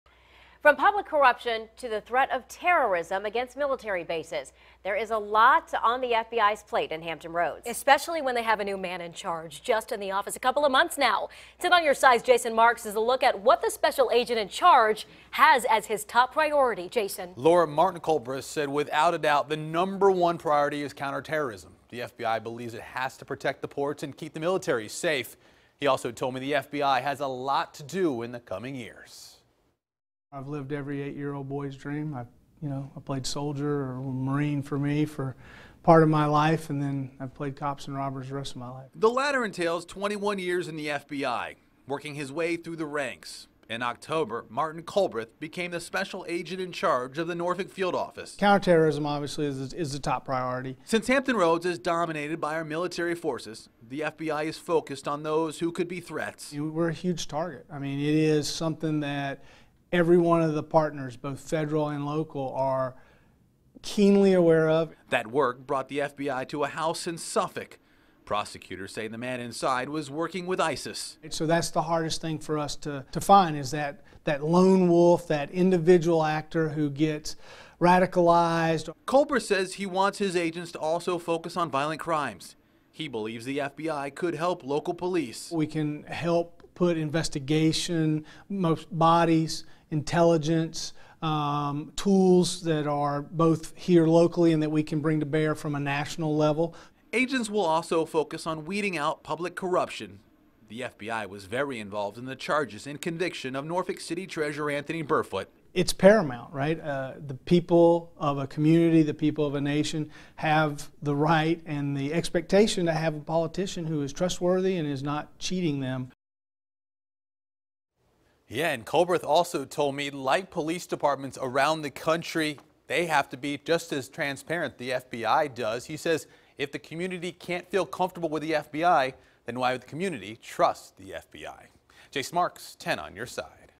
New head of Hampton Roads FBI office talks about challenges
Special Agent in Charge Martin Culbreth took over command for the Norfolk Field Office of the FBI in late October.